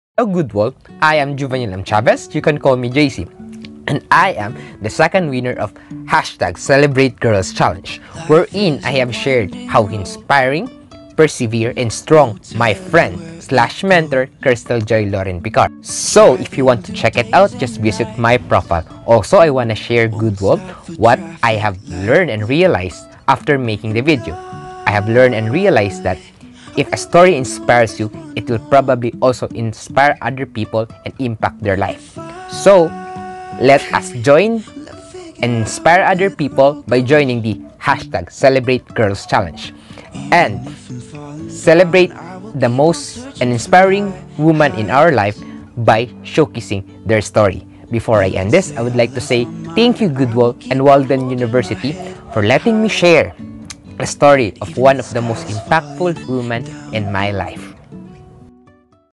I WON the CelebrateGirls Challenge in GoodWall
International Woman's Month is a months wherein we celebrate the amazing woman in our lives. In this video I have shared one of the most inspiring woman story in my life.